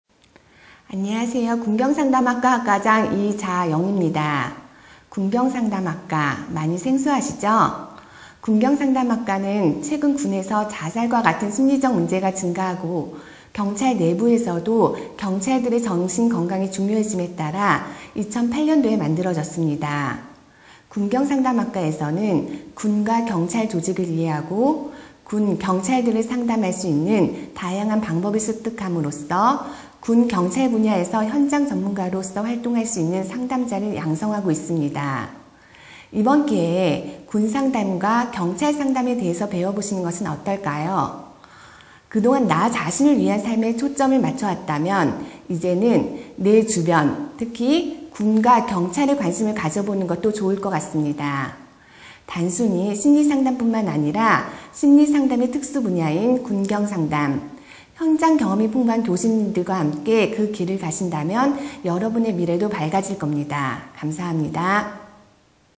[서울사이버대학교 심리상담학부] 군경상담학과 소개
안녕하세요~
서울사이버대학교 군경상담학과입니다.

2007년 제1회 교육인적자원부 원격대학 종합평가에서 최우수대학으로 선정된 서울사이버대학교 심리상담학부의 군경상담학과는 일반인(남·여) 또는 군 출신의 군내 전문심리상담관(군상담사) 양성 및 경찰 및 교도분야에서의 청소년·여성분야의 심리상담전문가 양성, 그리고 제대군인 및 경찰의 전직을 지원하는데 그 교육적 목표를 두고 국내 최초로 4년제 대학에 설립되었습니다.

서울사이버대학교에서 2014학년도 상반기 신.편입생을 모집합니다.
신입학은 고졸학력 이상이면 고교 내신이나 수능성적에 관계없이 누구나 지원 가능하고, 편입학은 학년별 학력자격만 충족하면 지원 가능합니다. 많은 지원 바랍니다.